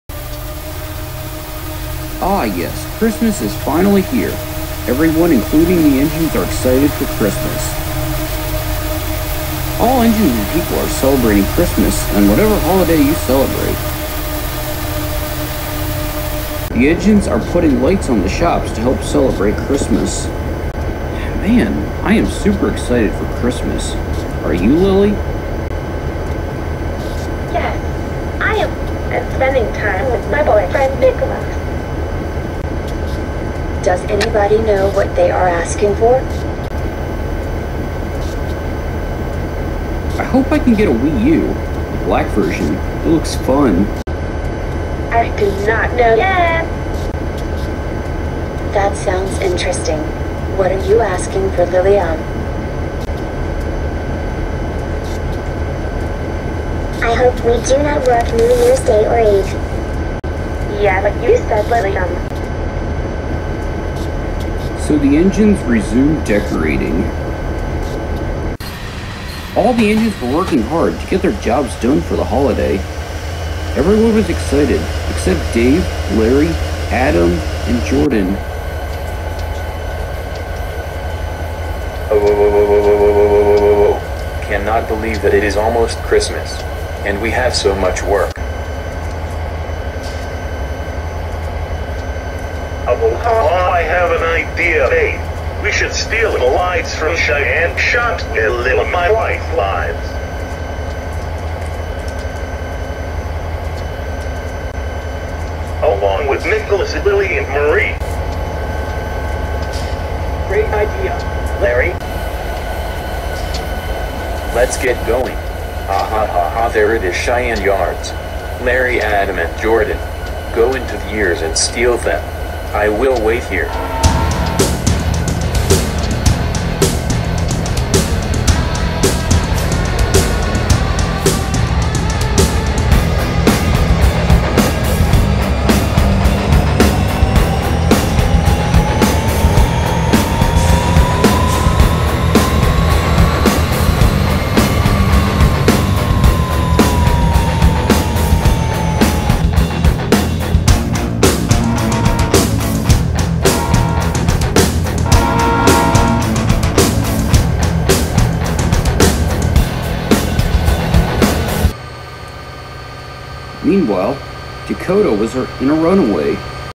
Rails of Sherman hill The Christmas Special! Part 1 ! First Half!
I created this video with the YouTube Video Editor Hi guys here is the First Half of 2016 Xmas Special ! Second Half is Next!